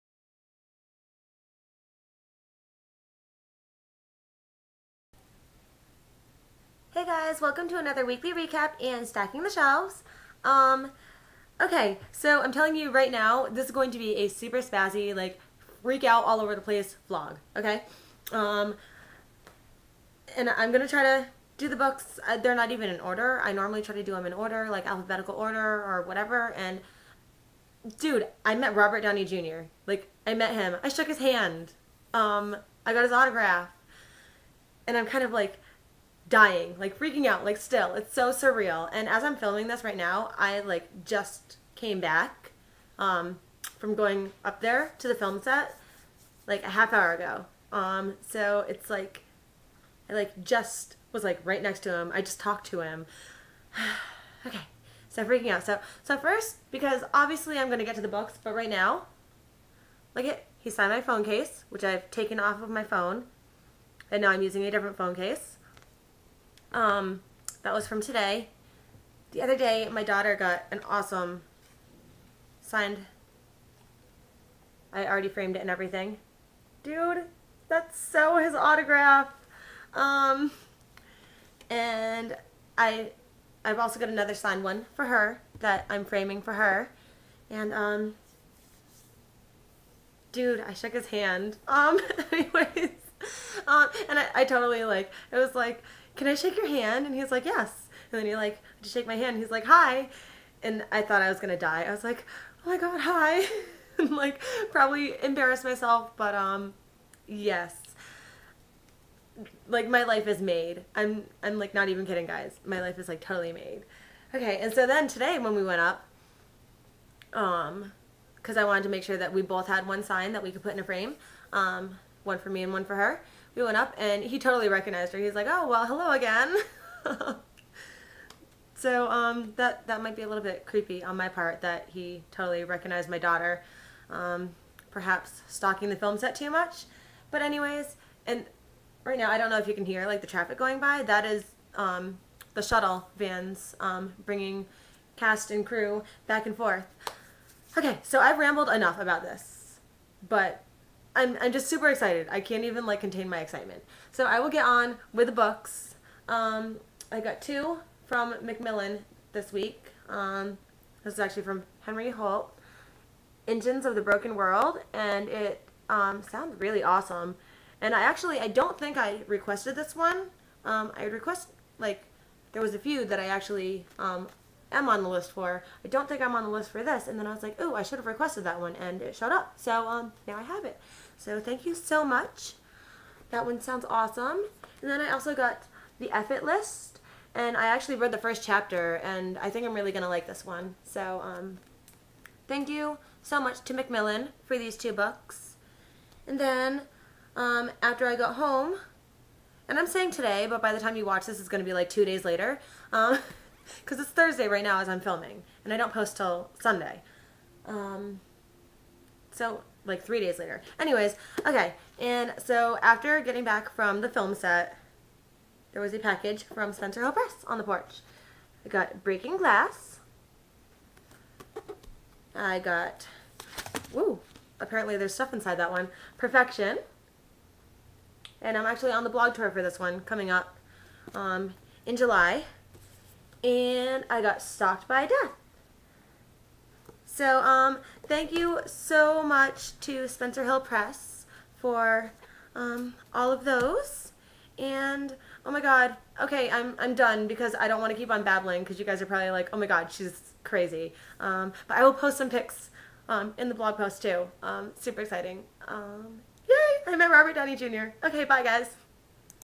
Weekly Recap / Stacking the Shelves (60)
Where I'm spazzing out because I met Robert Downey Jr. I do show what books I got though.